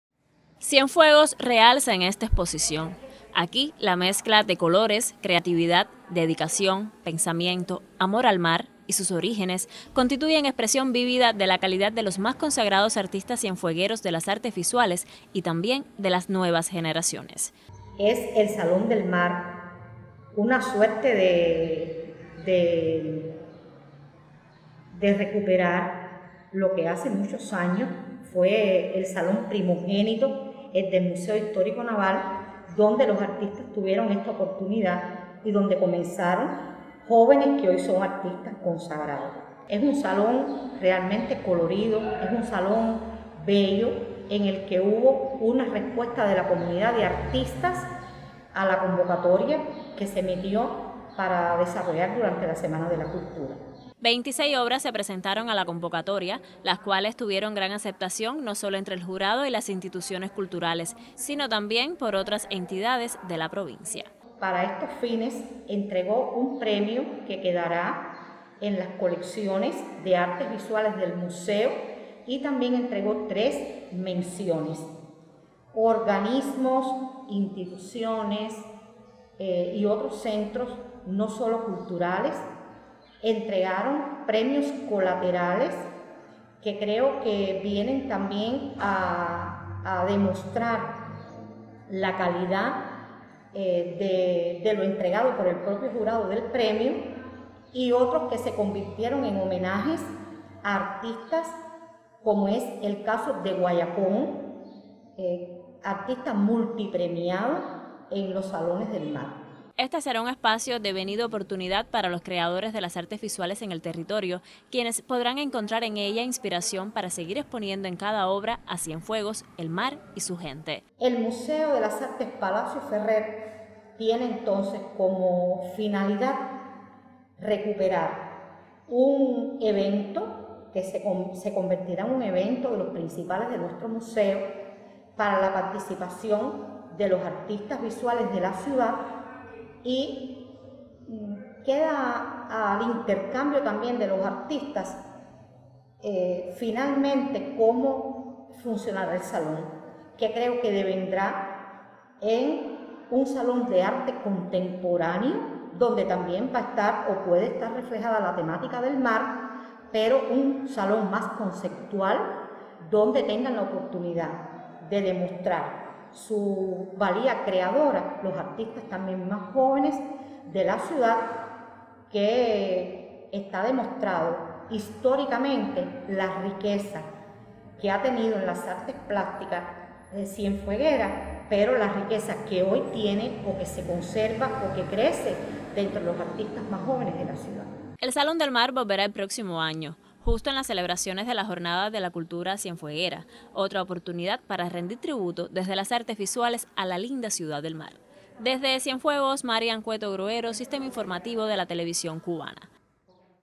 El Salón del Mar, abierto hasta el 21 de mayo
El “Salón del Mar” del Museo de las Artes Palacio Ferrer, estará abierto al público hasta el próximo 21 de mayo. Un espacio donde confluyen diferentes generaciones de artistas visuales de Cienfuegos desde lo tradicional hasta lo más contemporáneo.